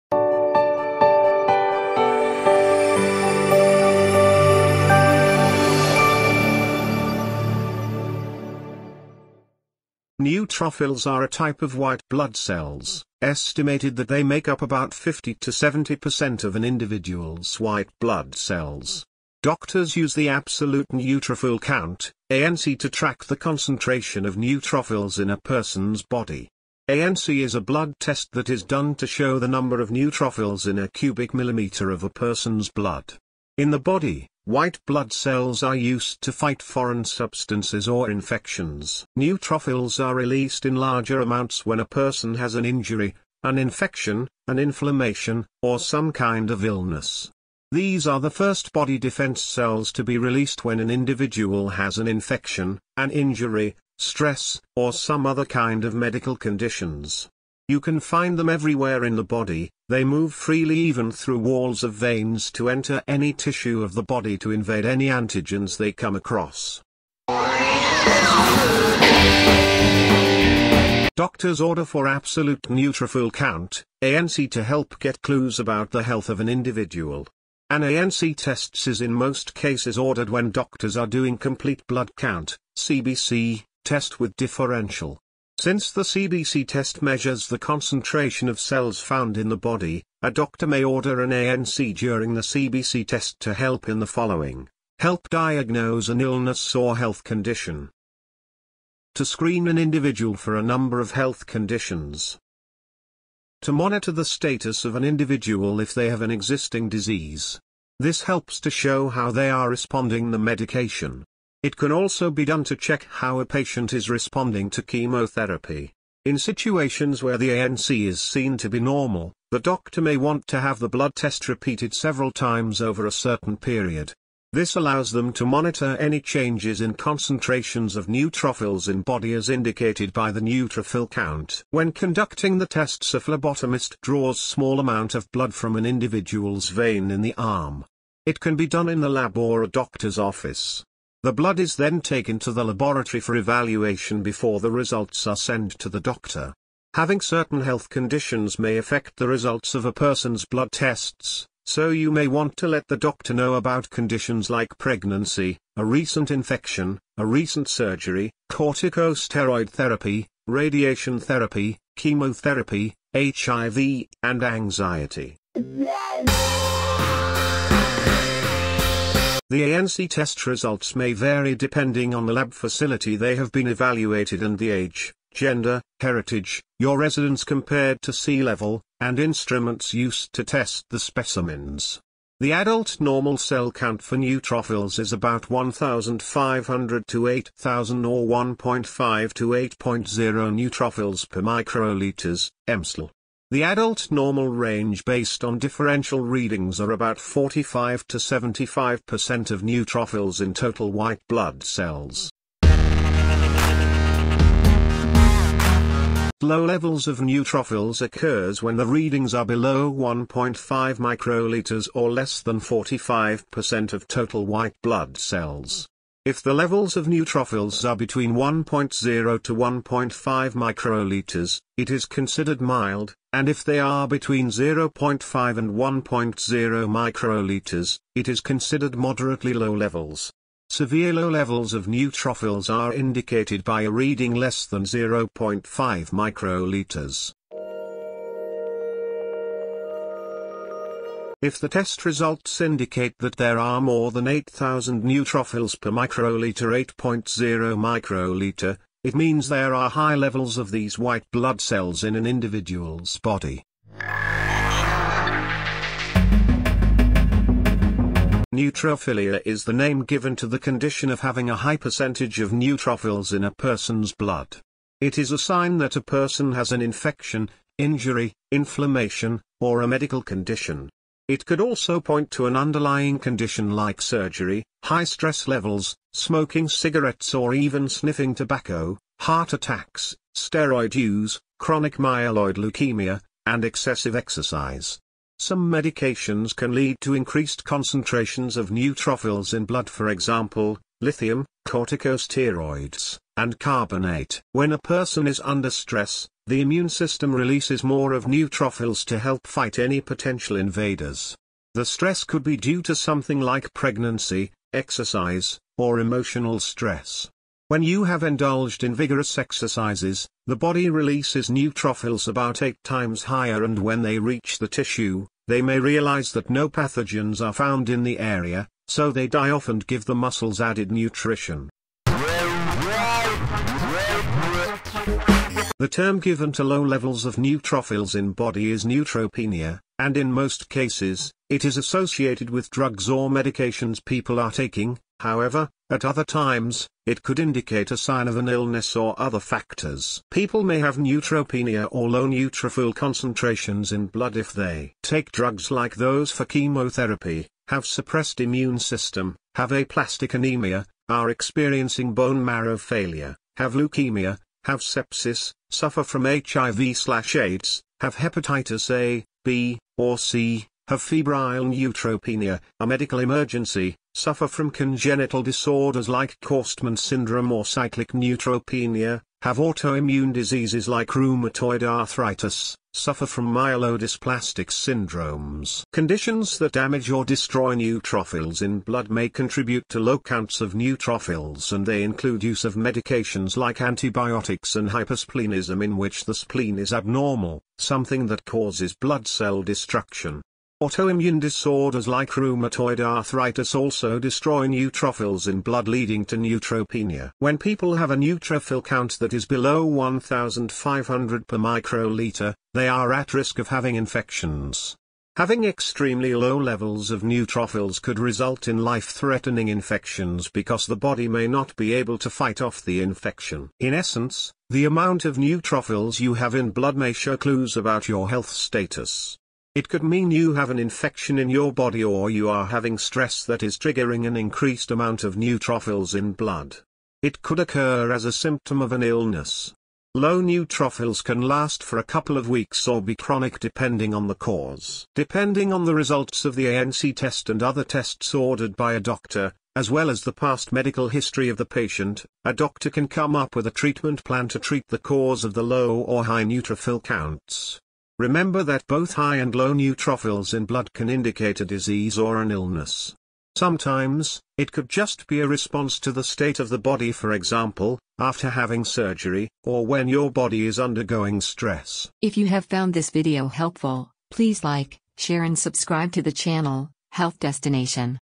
Neutrophils Absolute Count – High, Low (Causes), Normal Range
What does high neutrophils mean? What does it imply when the count is low? Find out about this important type of white blood cells that impacts our immune system.